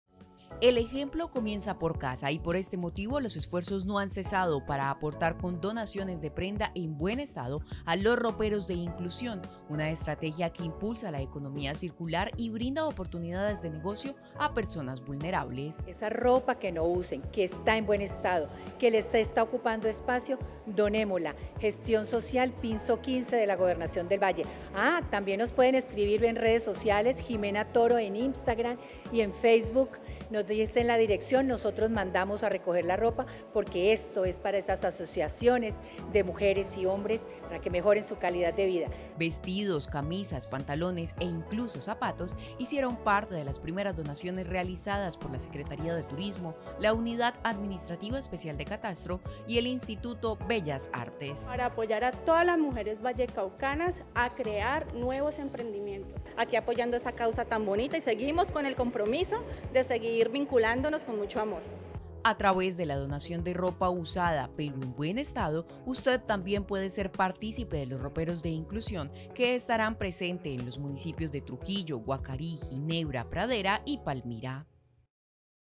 Comenzaron donaciones del Ropero de Inclusión
Son tres las entidades y secretarías del Valle del Cauca, las que ya comenzaron las donaciones para hacer realidad los Roperos de Inclusión en el departamento, iniciativa que apoya a madres y población vulnerable que busca emprender.